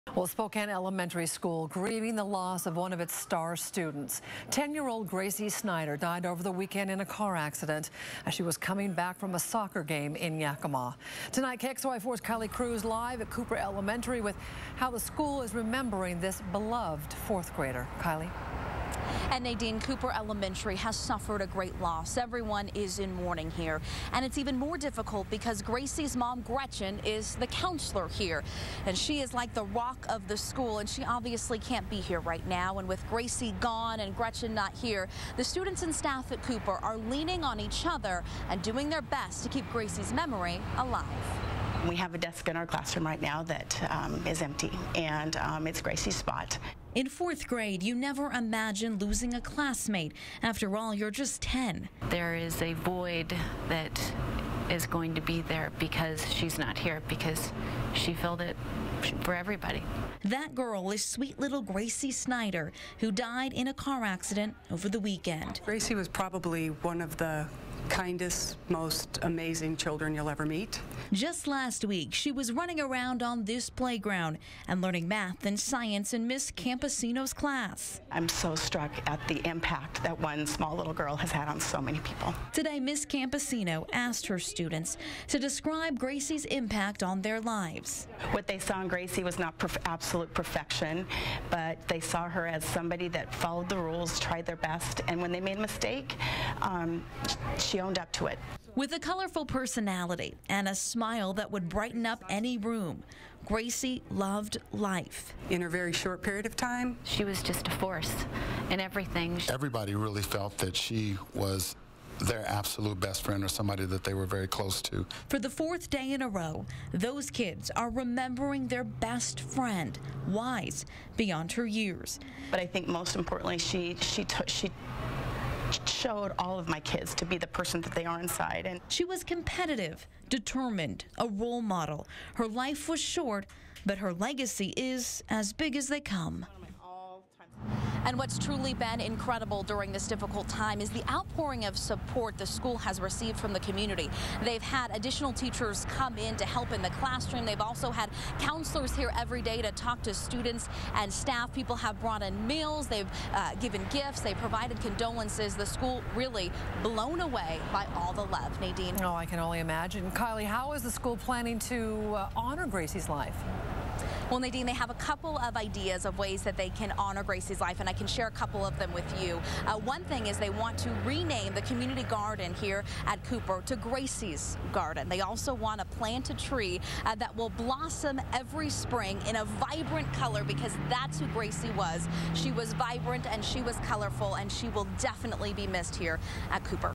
Cooper Elementary mourning loss of Gracie Snider
Cooper Elementary School is grieving the loss of Gracie Snider, a 10-year-old fourth grader at the school, a star student and soccer player who died in a car accident last weekend while coming home from a game in Yakima. KXLY4's Kylee Cruz reports.